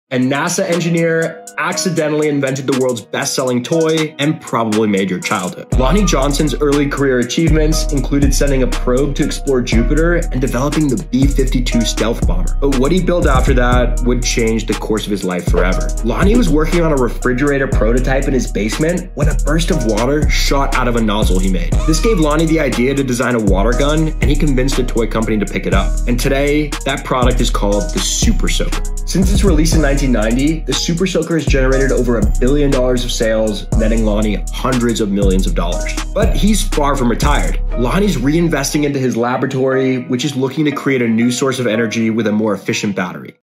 Lonnie Johnson's $1B Super Soaker🔫
Lonnie Johnson, a NASA Engineer, Created the $1B Super Soaker🔫